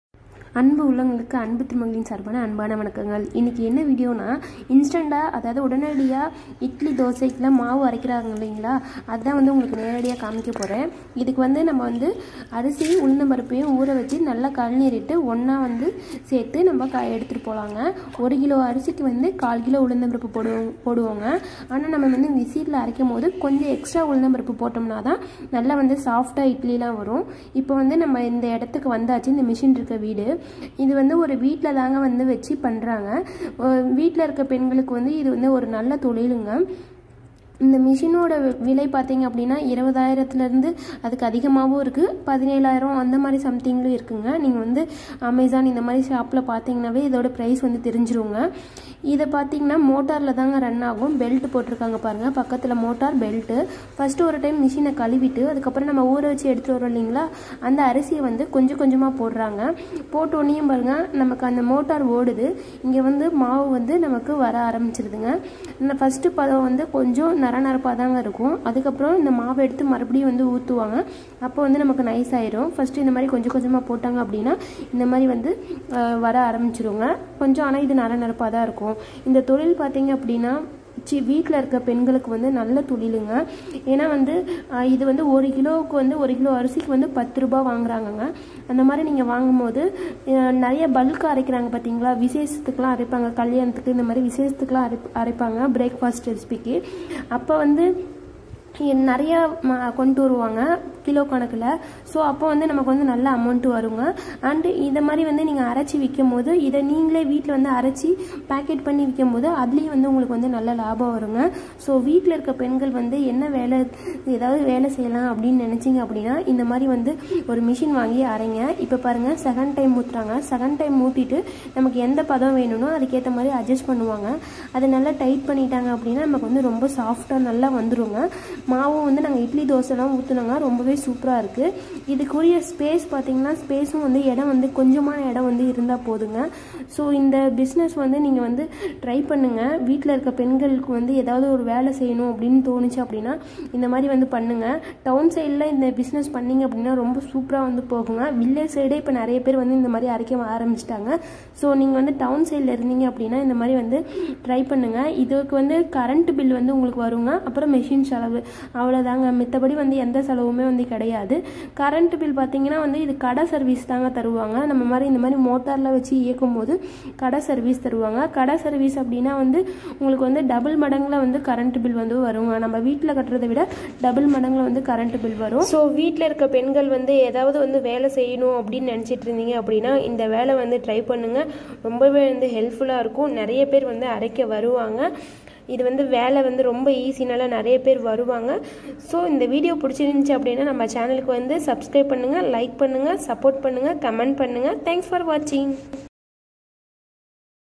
Instant Idly,Dosa,Iddiyappam maavu batter machine|small business idea at home|Anbu Thirumagal
about this video:small business idea for householders. it's quick and easily grinding the rice and dual.